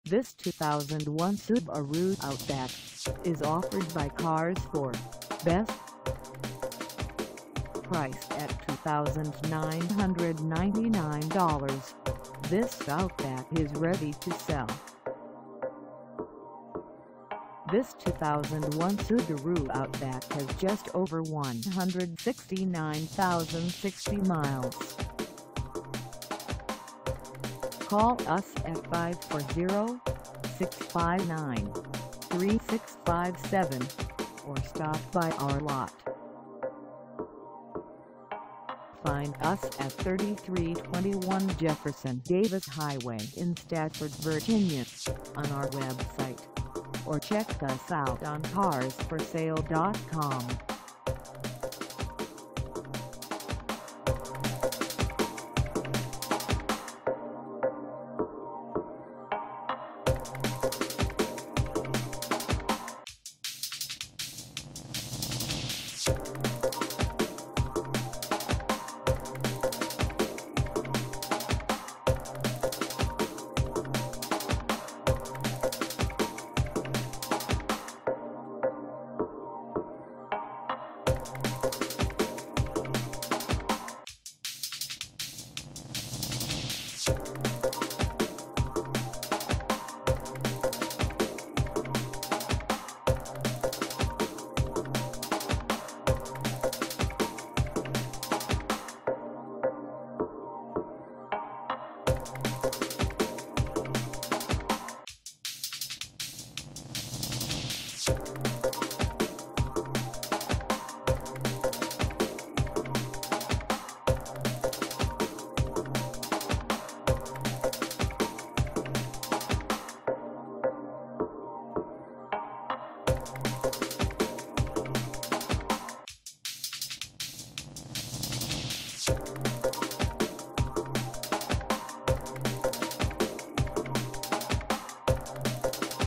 2001 Subaru Outback Base AWD 4dr Wagon w/Weather Pkg for sal
This 2001 Subaru Outback Base AWD 4dr Wagon w/Weather Pkg is for sale in STAFFORD, VA 22554 at CARS 4 BEST.
Engine: 4-Cylinder 2.5L
Transmission: Manual 5-Speed
Color: Unspecified
Mileage: 169060
Contact CARS 4 BEST 3321 JEFFERSON DAVIS HWY STAFFORD, VA 22554 to test drive this 2001 Subaru Outback Base AWD 4dr Wagon w/Weather Pkg today.